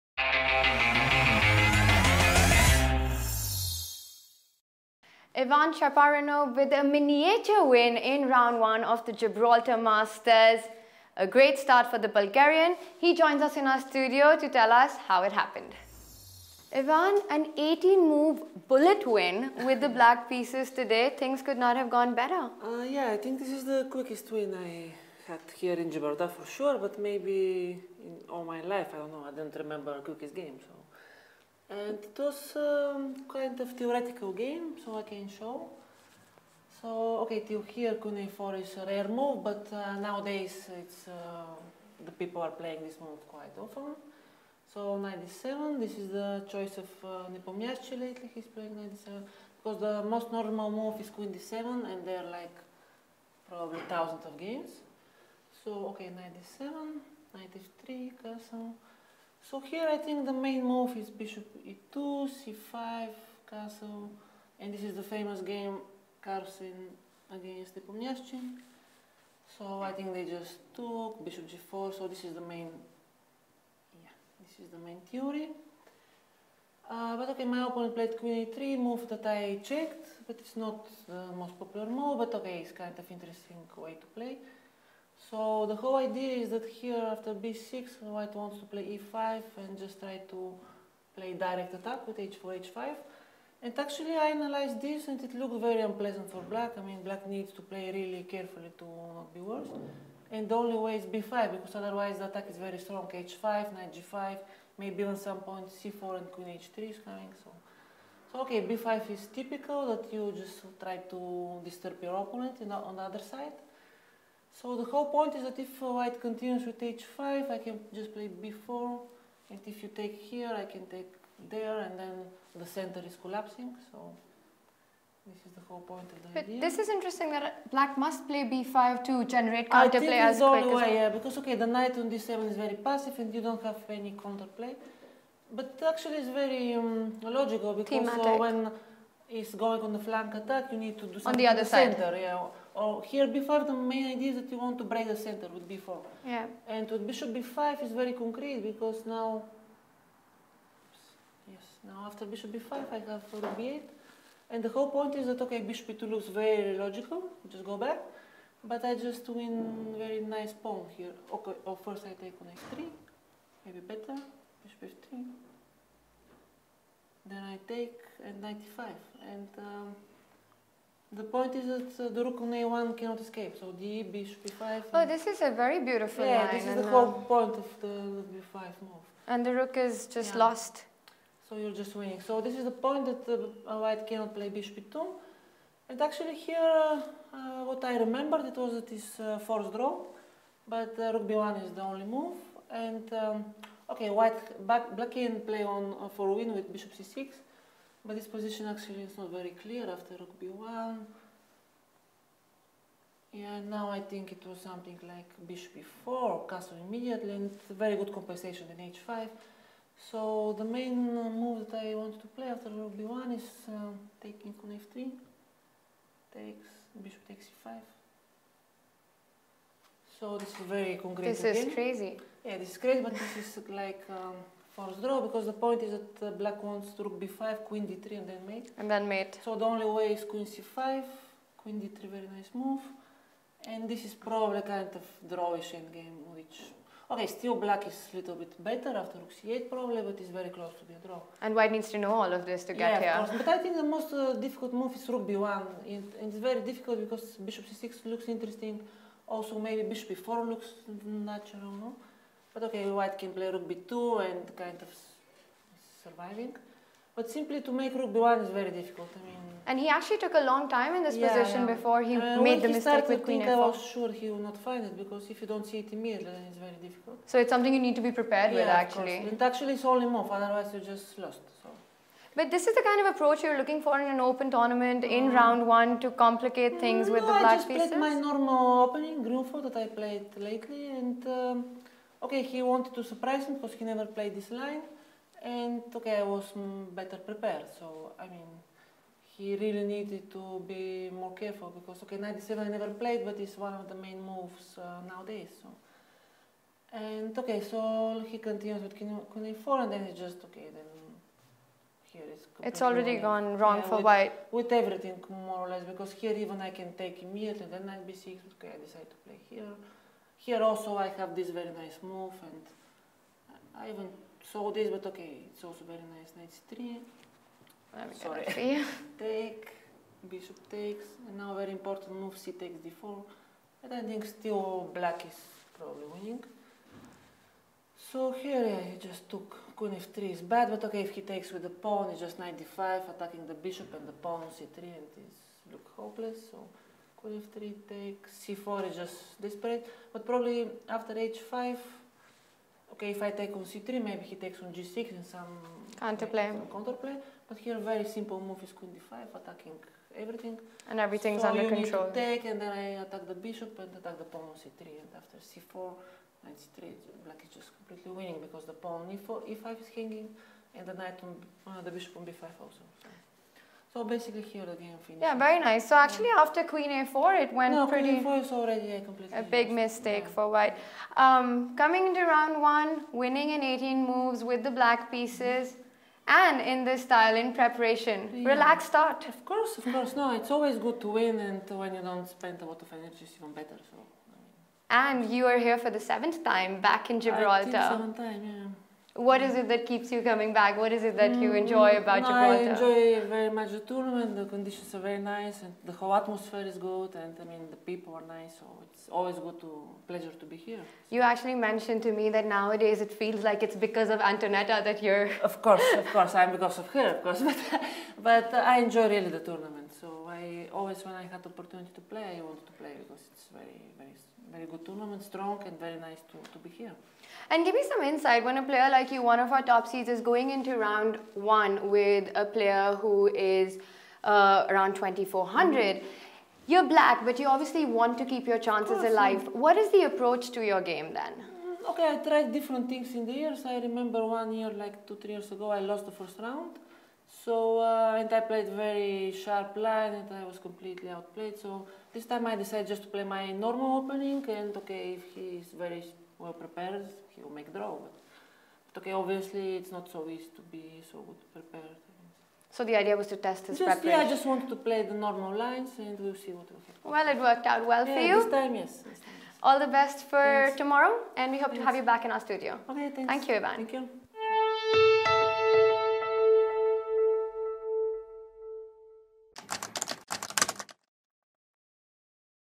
Round 1 GibChess interview with Ivan Cheparinov
An easy win for Ivan Cheparinov in Round 1 of GibChess